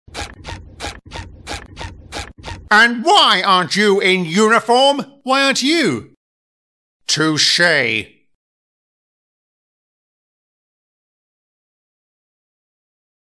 Sodor Short: Uniform
The Fat Controller is furious that Douglas is not in uniform...despite his own outlandish choice of wardrobe.